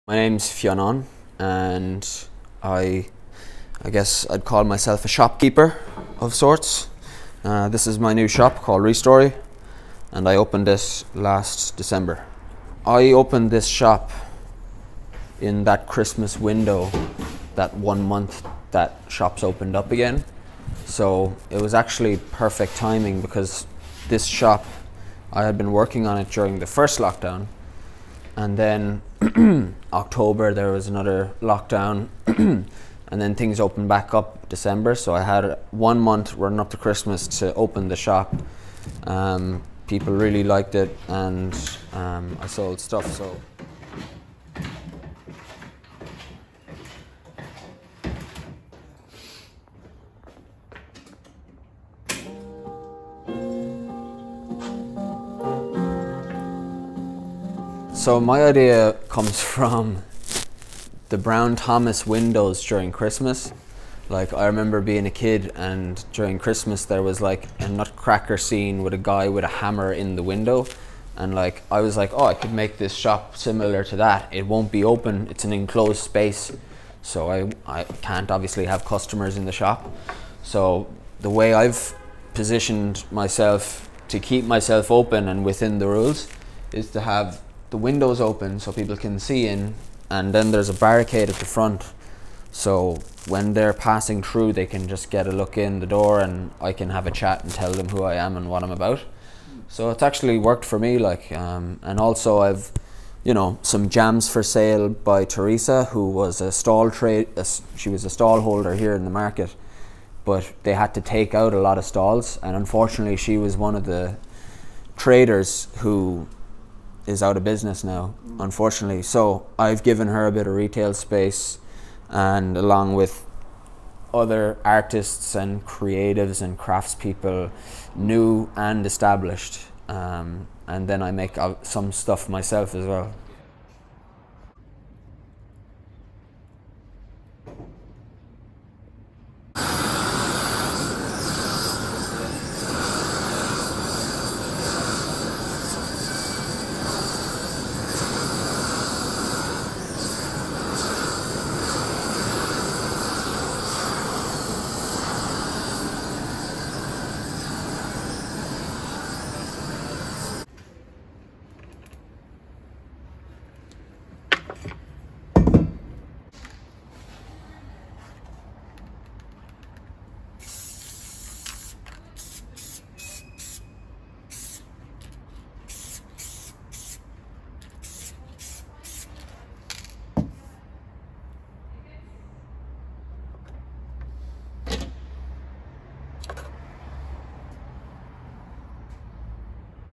Re: Story // Fionán Coughlan
Fionán Coughlan spoke to me about his new shop Re:Story.

The store, situated in Limericks Milk Market sells crafts made by local artists as well as Fionán himself!